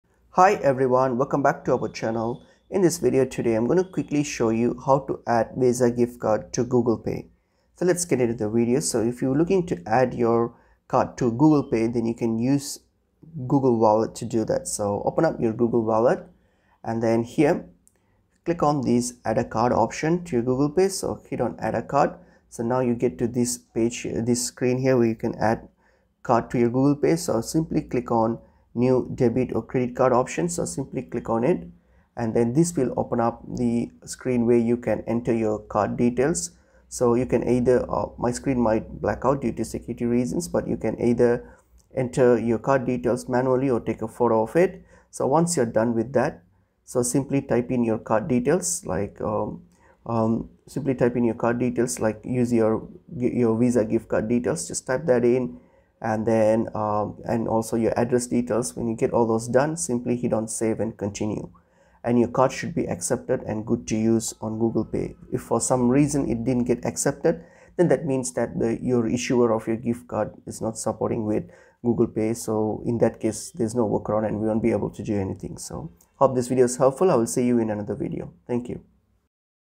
How to add visa gift card to Google Pay (Best Method)
In this video, I'm going to show you the best way to add a visa gift card to Google Pay. This is the easiest way to add a visa gift card to your Google Pay account, so you can easily pay with your phone!

If you're looking for a simple and easy way to add a visa gift card to your Google Pay account, then this is the video for you! By following these simple steps, you'll be able to add a visa gift card in minutes! So don't hesitate, watch the video and learn how to add a visa gift card to Google Pay!